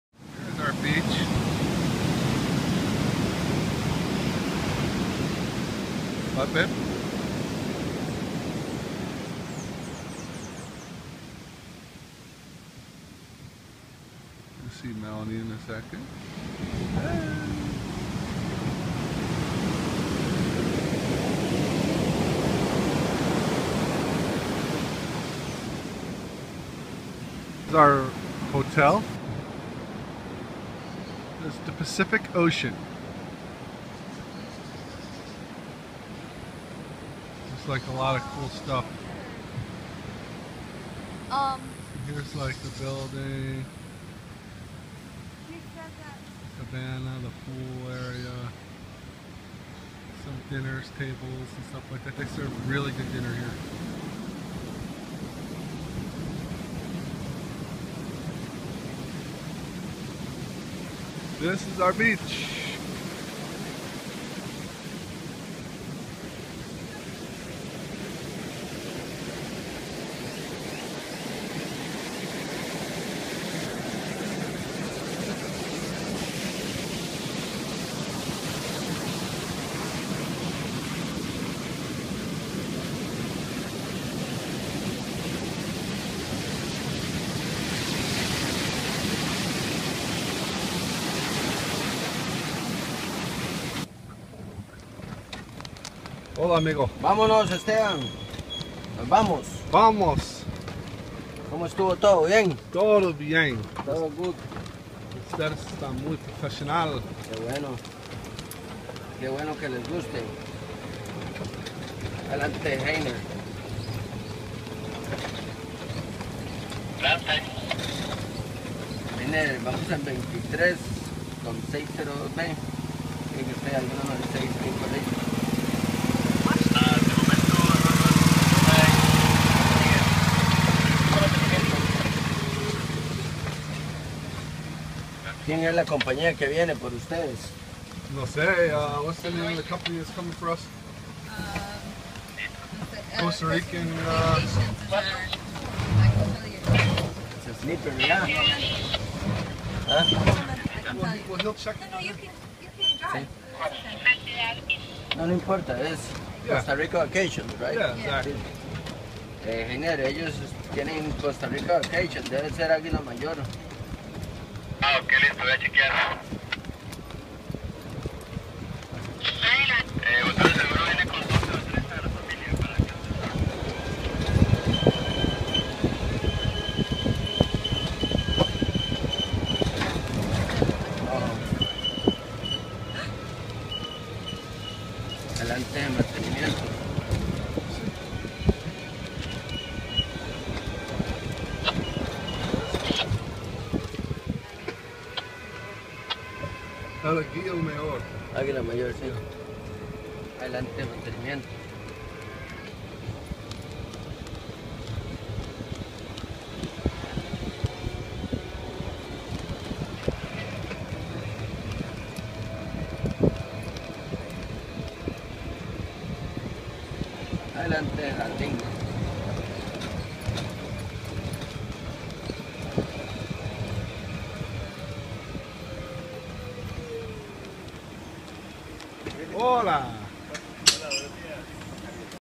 Arenas del mar, Costa Rica
Our honeymoon!! The people here were so professional, hotel was amazing and great food